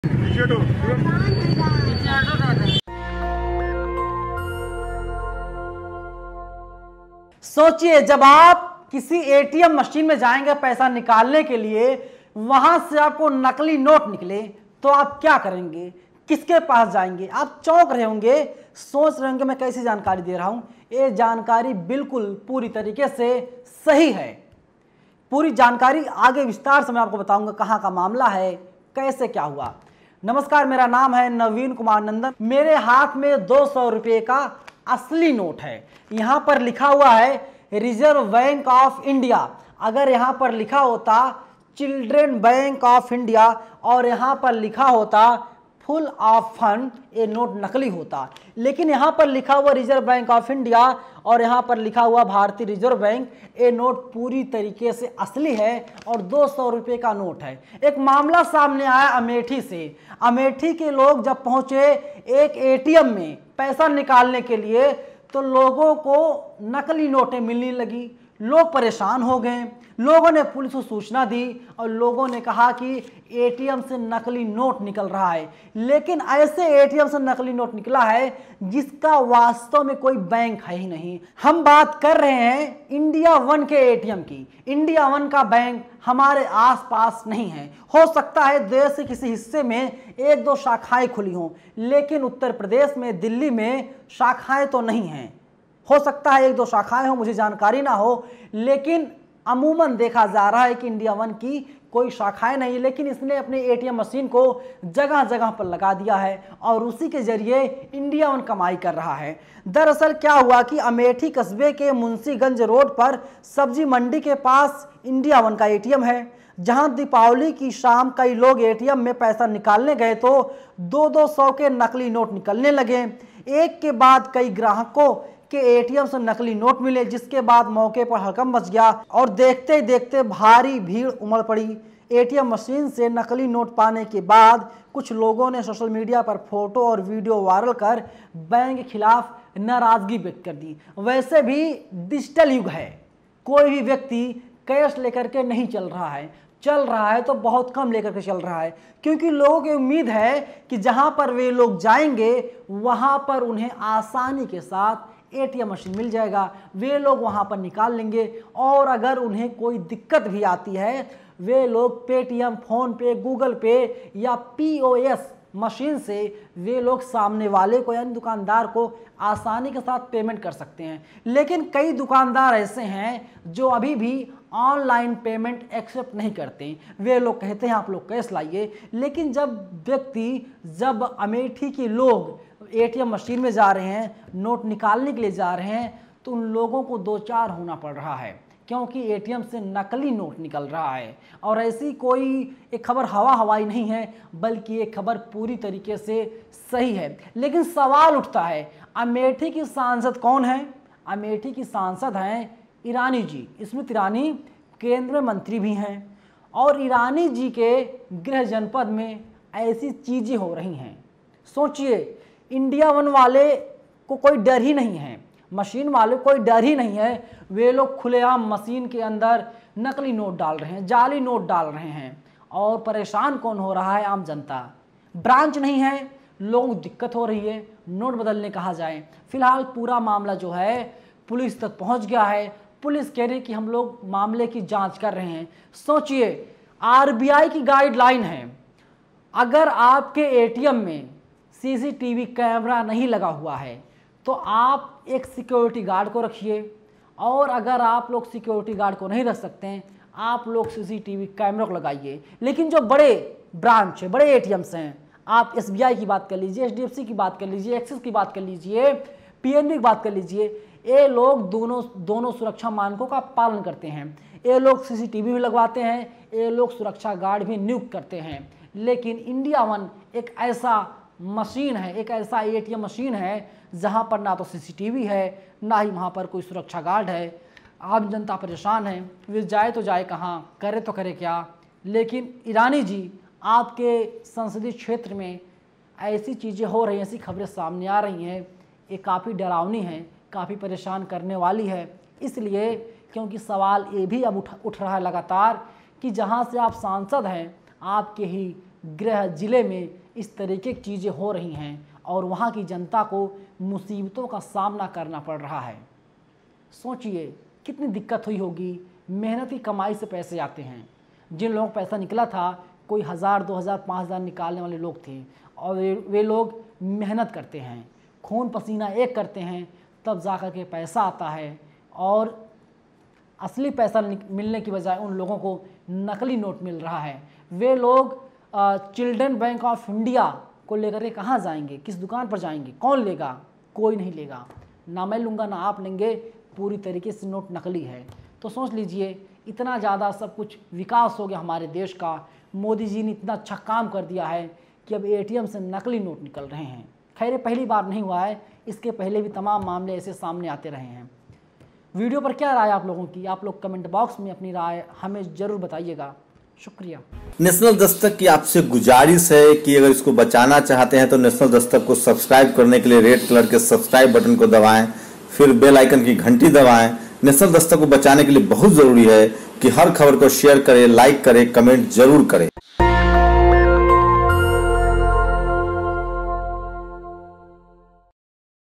नकली नोट पर स्मृति ईरानी चुप क्यों?/NAVEEN NANDAN ON FAKE CURRENCY IN AMETHI
Reporter-NAVEEN KUMAR NANDAN
Editor-MANUJ
Camera person-